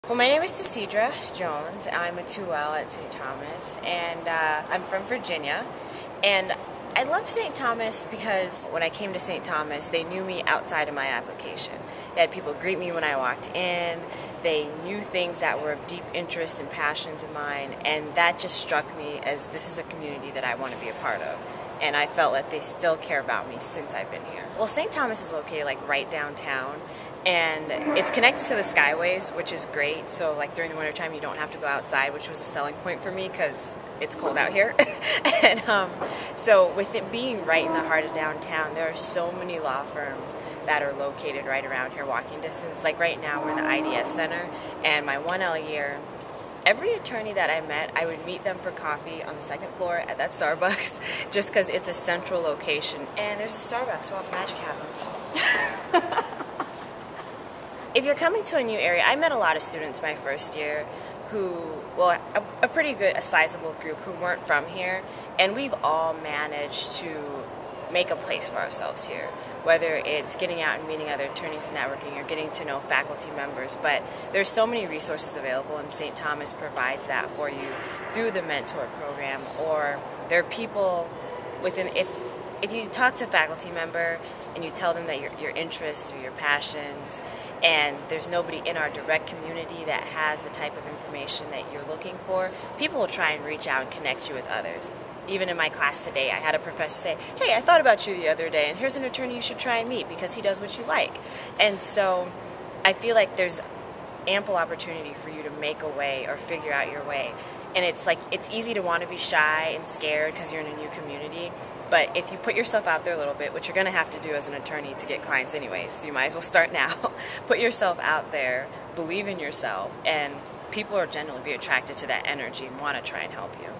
Why UST Law? Video Blog - Tisidra Jones (2L)
This video blog is for future and prospective UST Law students -- it is produced entirely by University of St. Thomas School of Law students.  Current 2L Tisidra Jones shares thoughts on making the move to Minnesota from Virginia, the UST Law community, and the law school's prime Minneapolis location for discovering mentors.